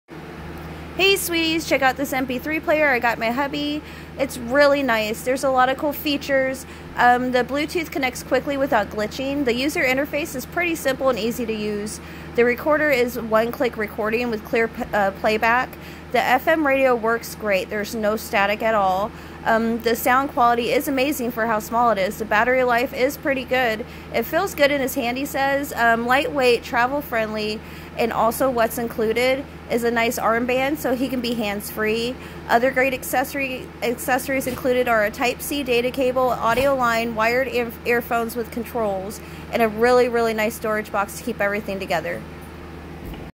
MP3 Player with Accessories
Hey sweeties! Check out this MP3 Player I got my hubby! It's really nice! Alot of cool features!  The bluetooth connects quickly without glitching! The user interface is pretty simple and easy to use! The recorder is 1 click recording with clear playback. The fm radio works great! No static at all! The sound quality is amazing for how small it is! The battery life is pretty good! Feels good in his hand, lightweight, travel friendly! Also what's included is a nice armband so he can be hands free! Other great accessories included are Type C data cable, audio line, wired earphones with controls and a really nice storage box to keep everything together! This is a stylish, powerful MP3 Player that's affordable!